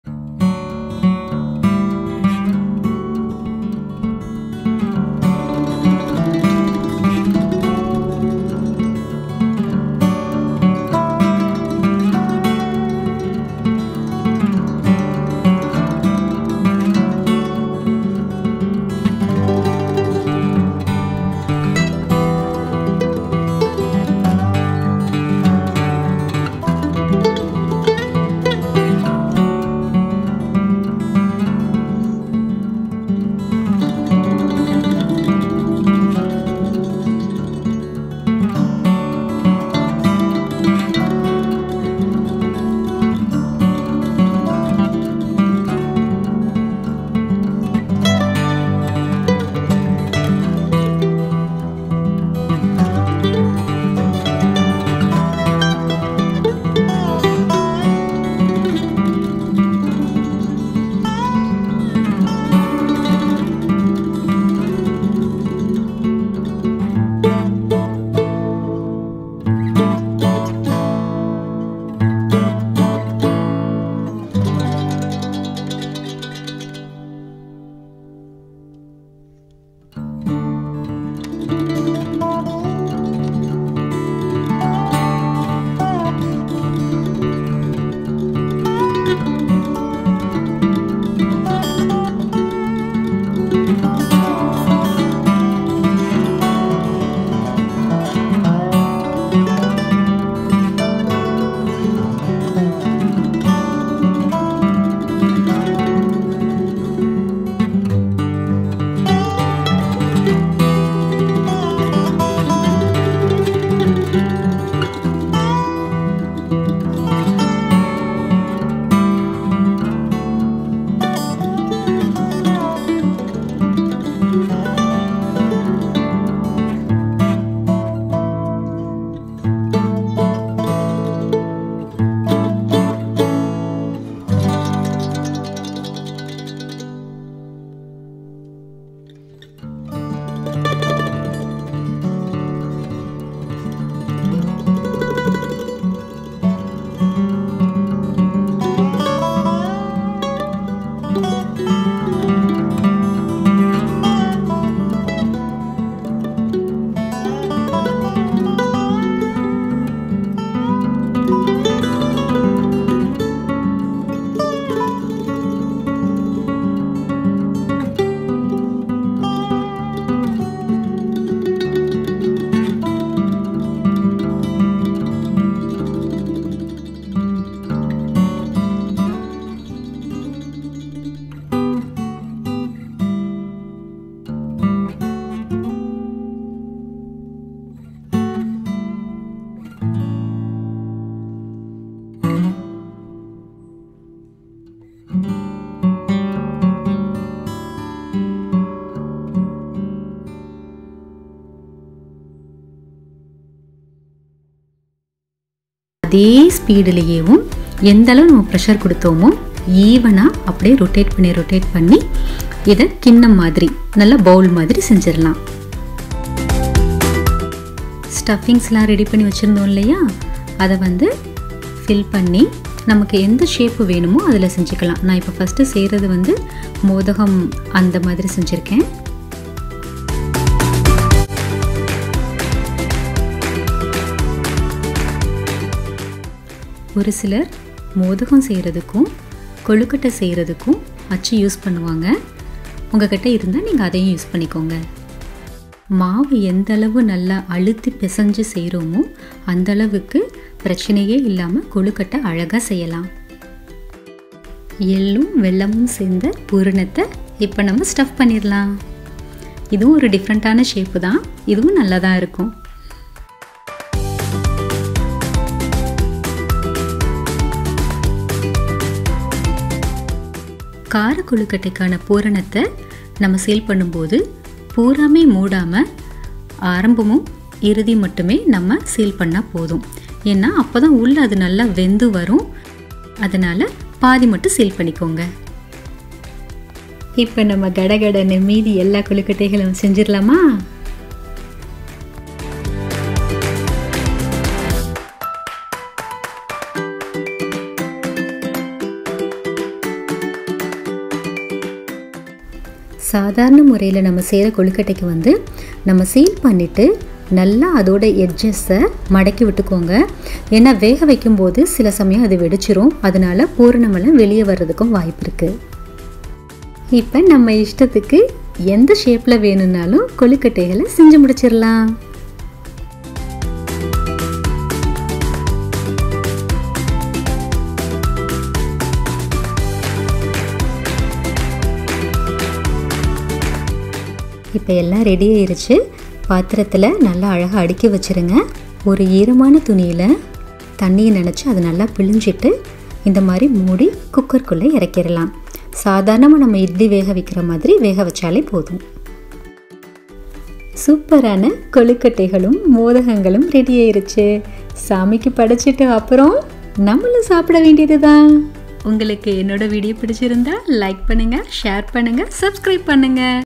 How to make Modak perfectly | 3 stuffing | vinayakar chathurthi special
Happy to share this "Bowl making" trick, which I learned at the age of 10 from my Appa. Thank you Daddy for the wonderful tips and tricks 😘...

Notes:

* For sesame stuffing, you need to dry roast them first

* For kaara kozhukattai (spicy stuffing), add 🍋 juice. For extra softness, add a pinch of baking powder...
kolukattai
பூரண கொழுக்கட்டை
poorana kolukattai
ellu kolukkattai
paruppu kolukattai
thengai kolukattai